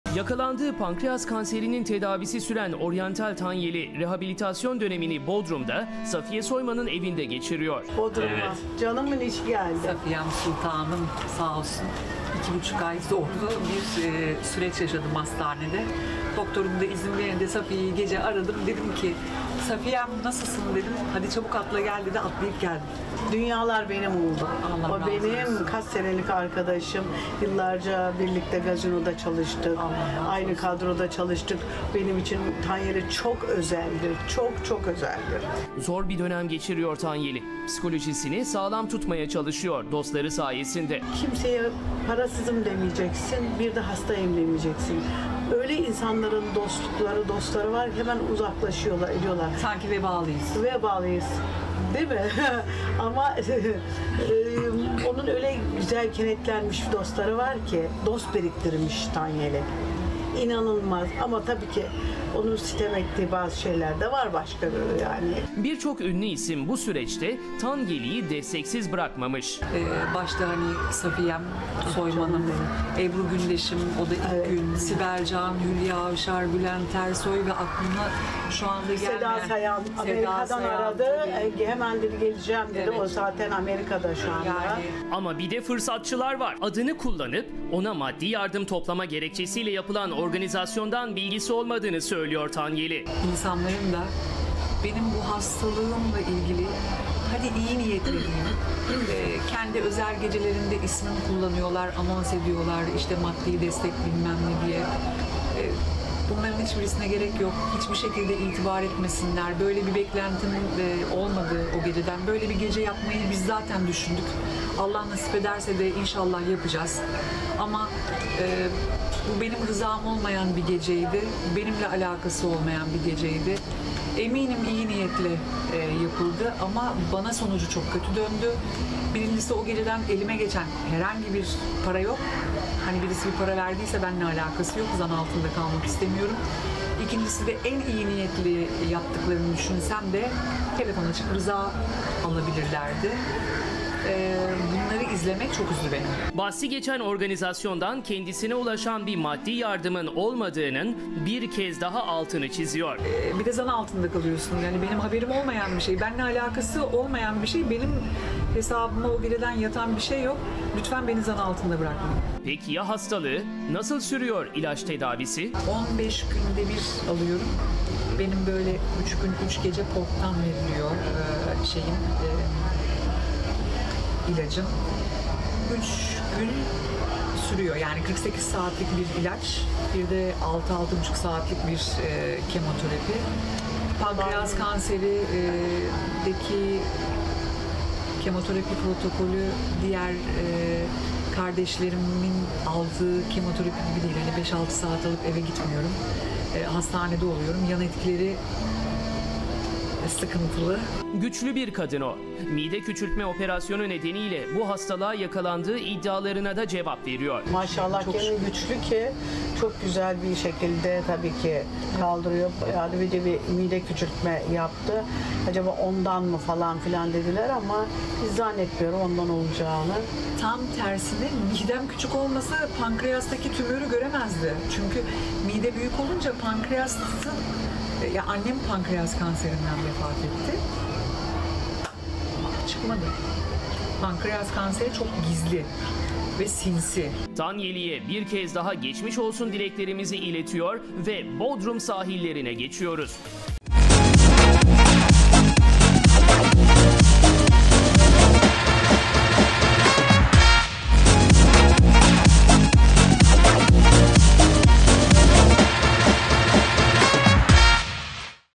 Kanser tedavisi gören Tanyeli'ye Safiye Soyman'dan destek!
Bodrum'da Safiye Soyman ile kalan Tanyeli açıklamalarda bulundu.
Adını kullanıp yardım gecesi düzenleyen dostlarına sitem eden Tanyeli, 'Ben kimseden böyle bir talepte bulunmadım, bana gelen para da yok' diyerek hayal kırıklığını anlattı!